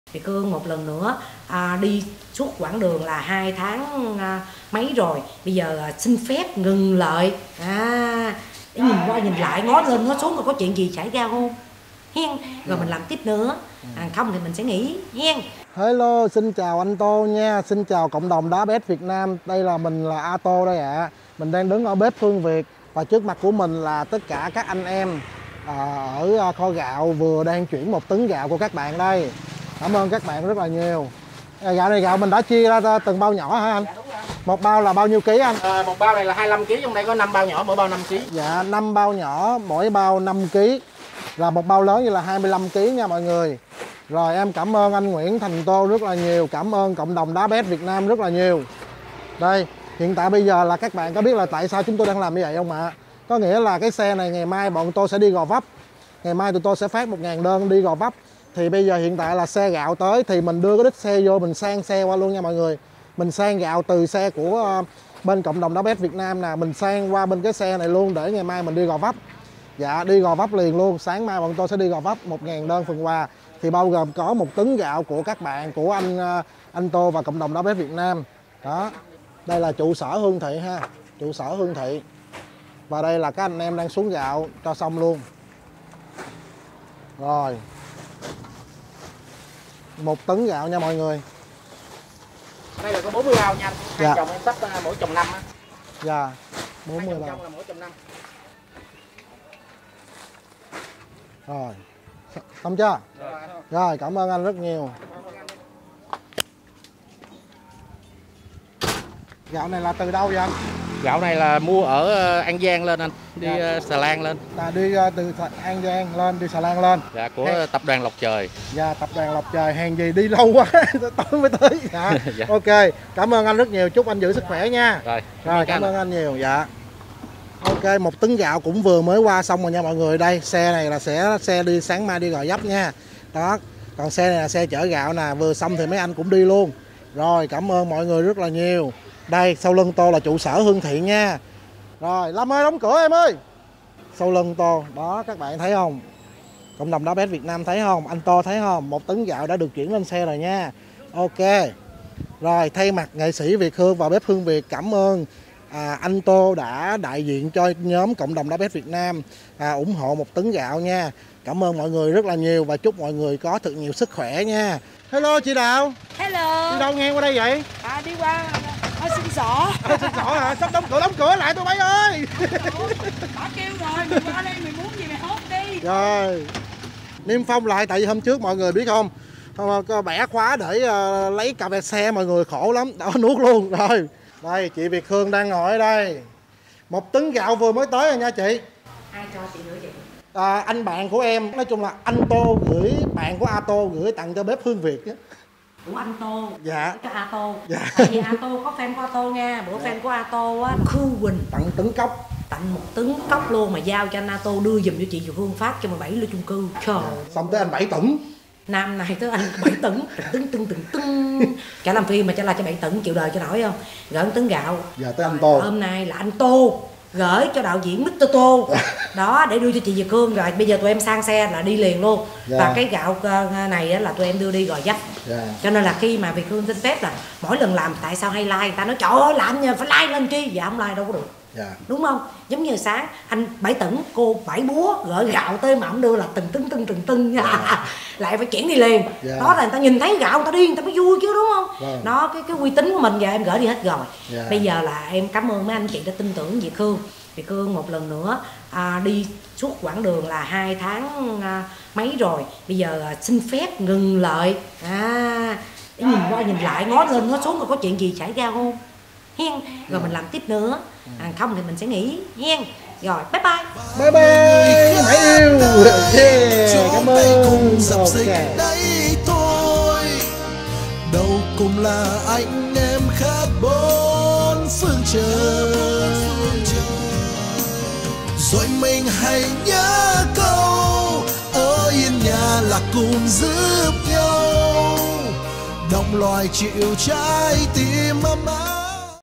Nghe tin Việt Hương NGƯNG LÀM TỪ THIỆN cộng đồng ĐÁ PES VIỆT NAM chuyển GẤP 1 TẤN GẠO ngay trong đêm
Việt Hương thay mặt Bếp Hương Việt và bà con xin cảm niệm công đức đến cộng đồng ĐÁ PES VIỆT NAM. Xin cảm ơn các bạn tình nguyện viên đã hỗ trợ. Chúc mọi người có thật nhiều sức khỏe, bình an.

MỘT VIỆT HƯƠNG KHÁC BIỆT TRONG NHỮNG NGÀY DỊCH
Mỗi ngày nhận hàng trăm cuộc gọi, tất bật từ sáng đến tối với gạo, mắm, nước tương, rau củ, bánh ngọt, Việt Hương tâm sự cô cảm giác như đang làm nghề khác.

Ngân hàng : MB BANK
Chi nhánh : Q10
Chủ TK : TO CONG TUAN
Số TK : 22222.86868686